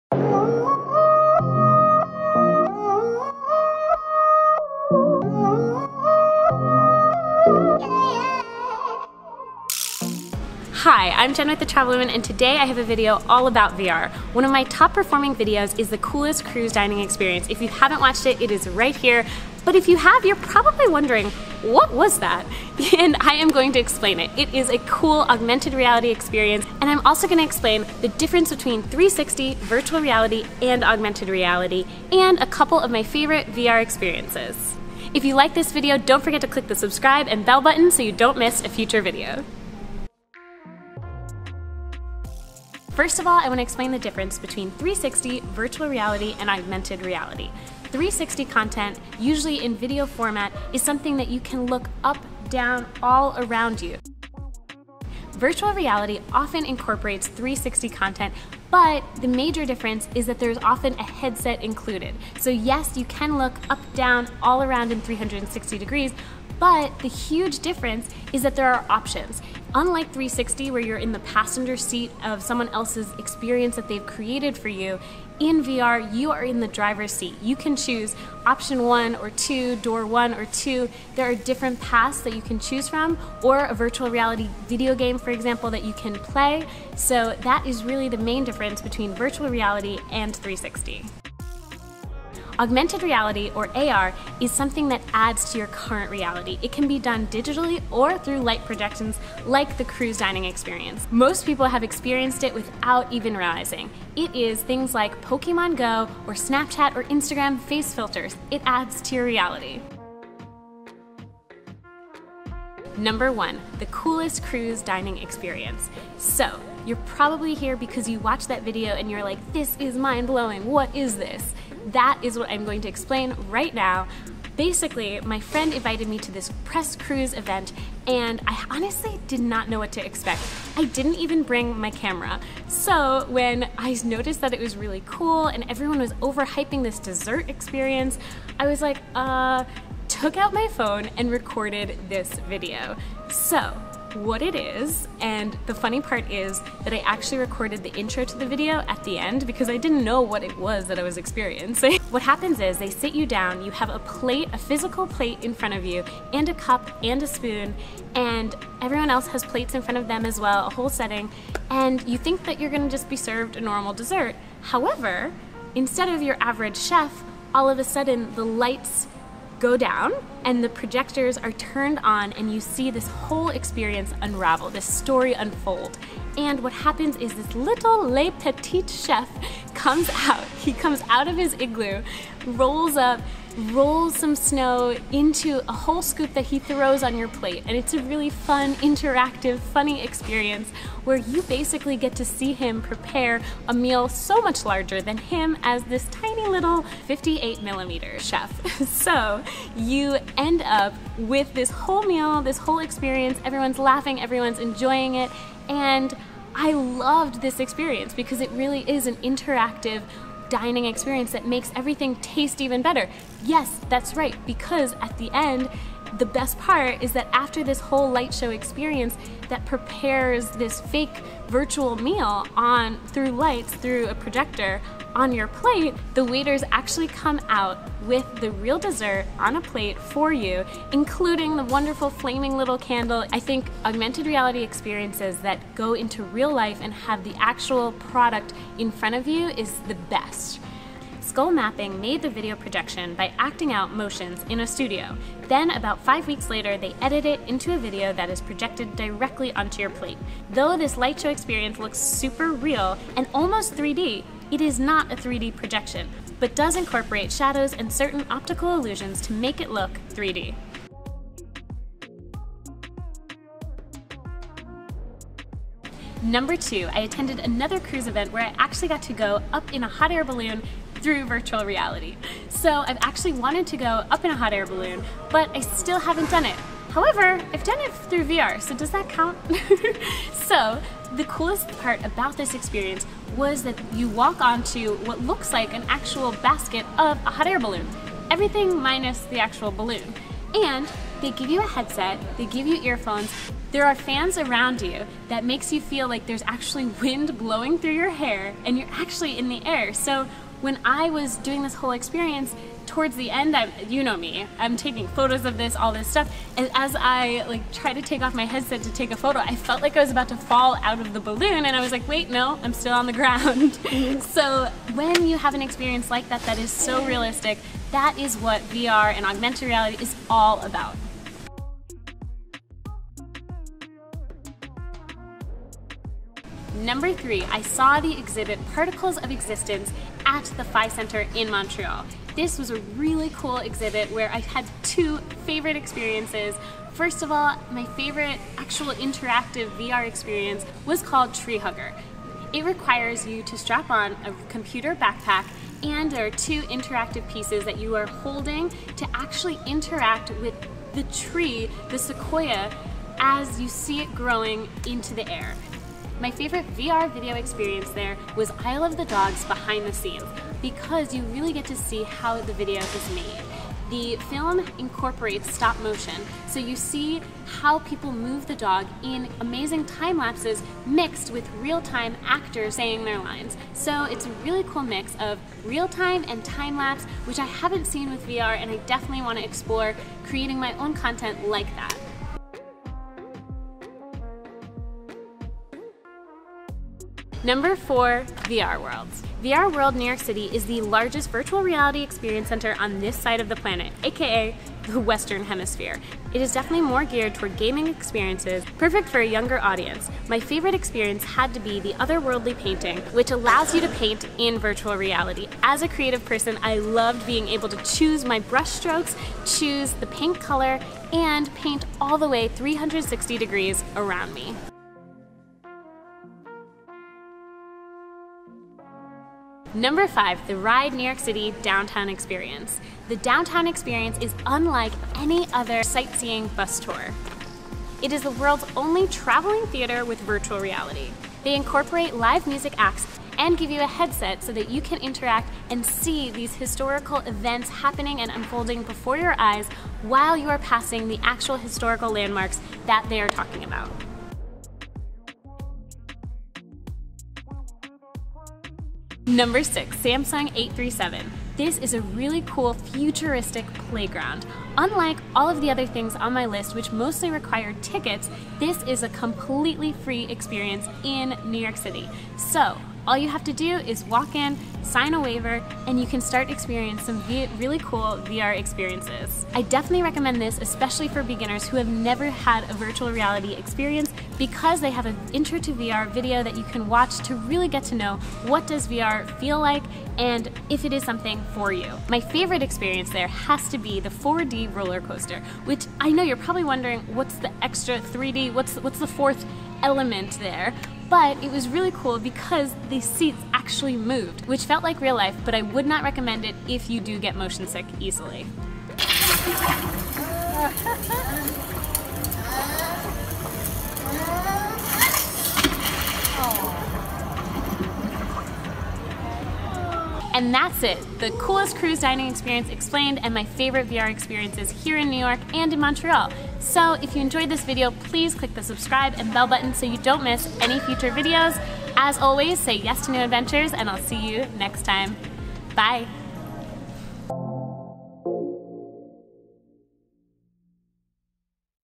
Coolest Cruise Dining Experience Celebrity Edge EXPLAINED
Le Petit Chef by Skullmapping explained: from the type of projection to the optical illusions that make it look 3D and the exact size of this tiny chef, learn what makes my "Coolest Cruise Dining Experience: Celebrity Cruises" video my top performing video!

3. Liquid Nitrogen Ice Cream
5. Guide to SOHO, NYC